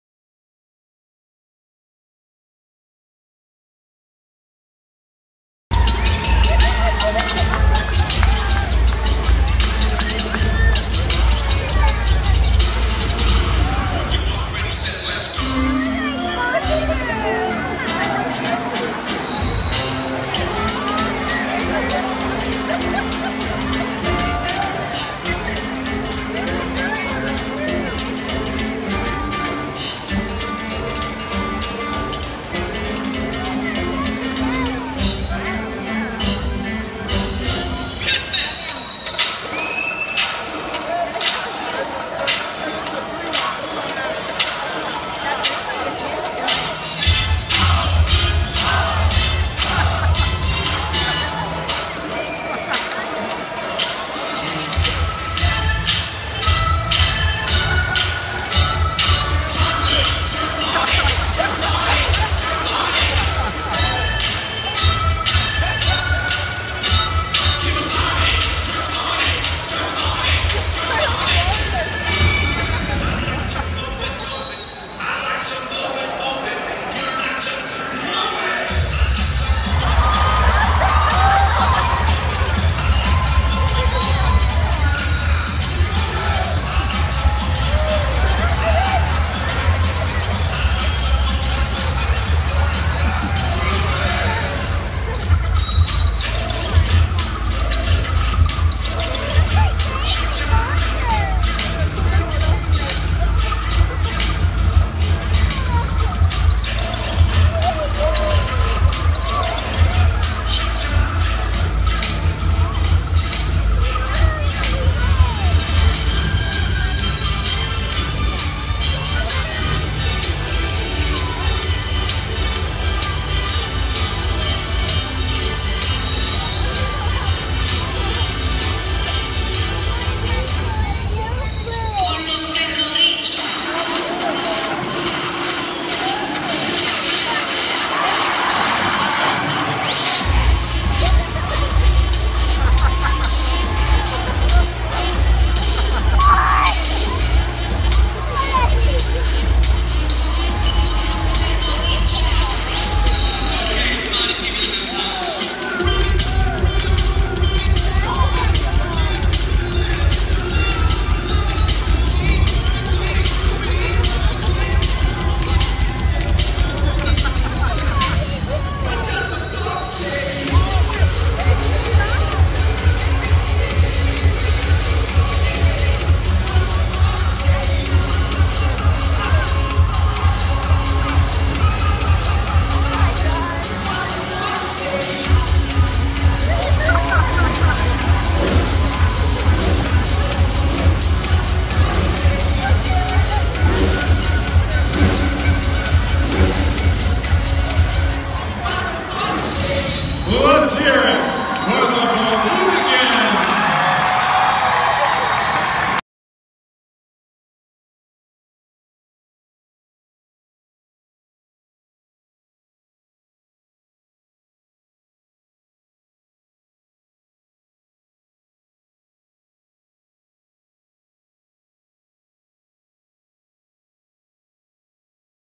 CORPUS CHRISTI SHRINE CIRCUS 2014 SKIT 3.. THIS IS HILARIOUS!!!
THIS HAD ME LAUGHING HELLA HARD... SO FUNNNY

HOPE YALL ENJOYED THIS AS MUCH AS ME AND MY Family did.. THANKS EVERYONE